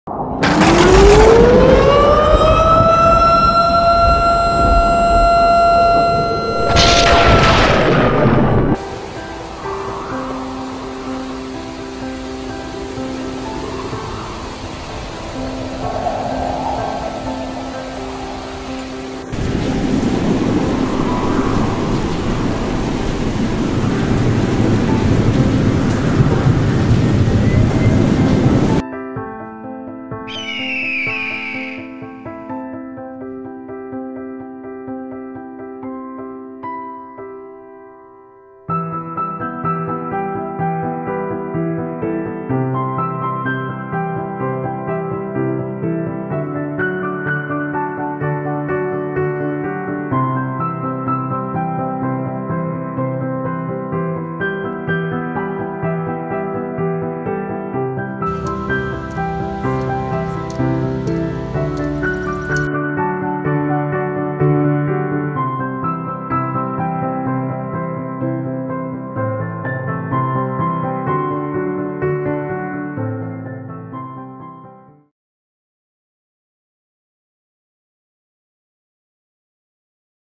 Hurricane Proof Telescoping Flagpole | Atlantic Flag Pole
WOW this telescoping flag pole can withstand hurricane force winds! And it's made in America, talk about taking a STAND with your flagpole.